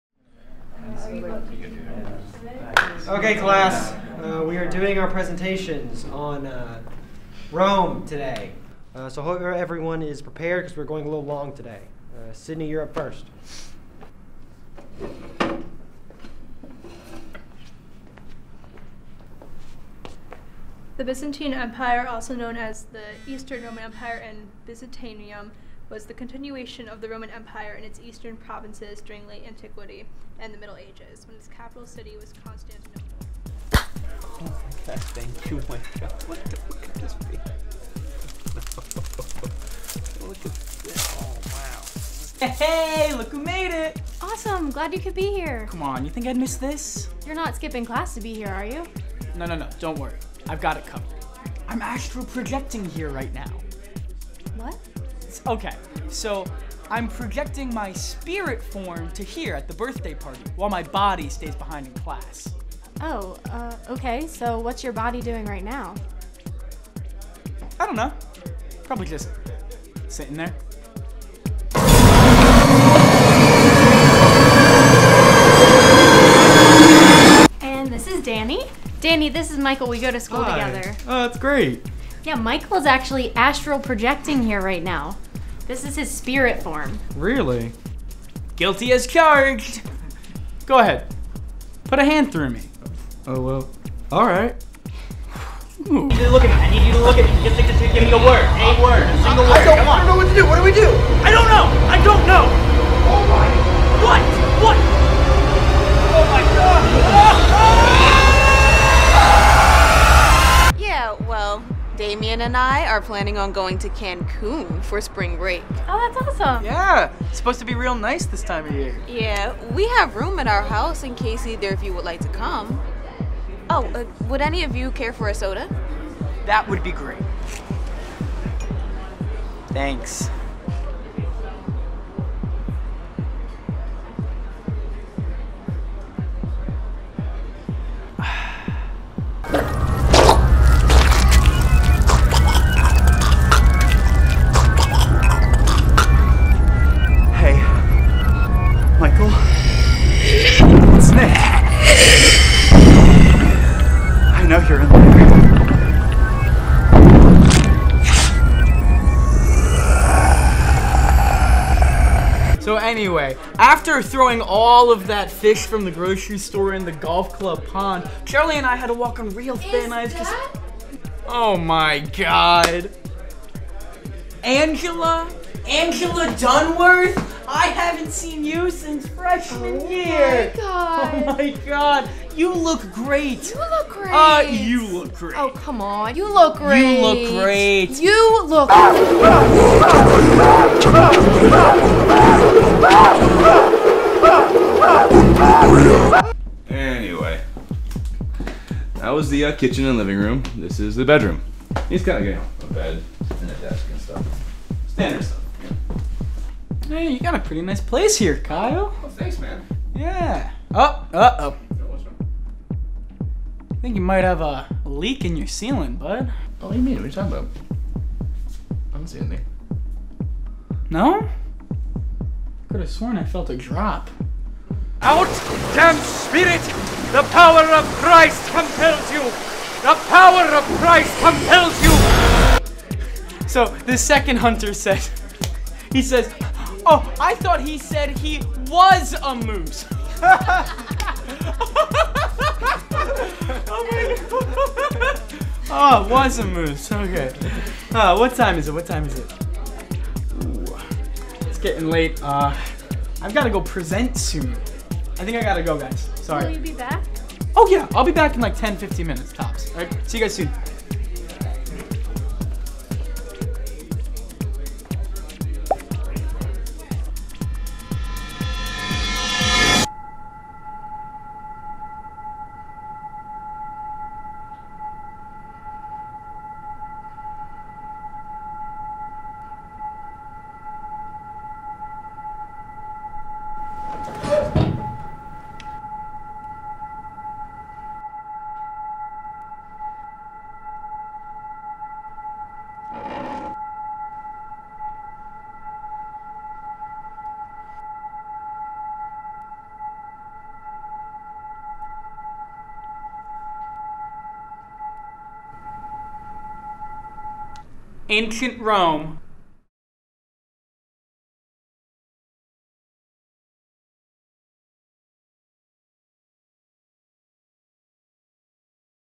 Classtral Project - Temple SMASH
A student knows the perfect loophole for skipping class.

Written by: Devin O'Brien and Brian Diroff
Directed by: Brian Diroff and Eddie Bick

Starring:
Devin O'Brien
Brianna Williams
Nick Eiser
Zack Martin
Julie Fosco
Ray Hopkins
Daniel Kilderry
Imani Jones
Kyle Lehner
Nikolas Collins
Sydney Turturro
Alex Nicoletti
Brian Diroff
Eddie Bick
Camille Hernandez
Paul Burke
Cary Harrison
William Shires
Ian Applebaum

Producer: Kyle Carozzi
DP: Daniel Silver
Editor: Joe Lorenzo
Audio: Mike Moscarelli, Dan Fare, Cameron Toft
AD: Emily Stine
AC: Luciano Cecchi
Grip: William Shires
PA: Ben Marino
PA: Jessica Shelton
PoDN: Jaclyn Silvestri
EP: Sydney Turturro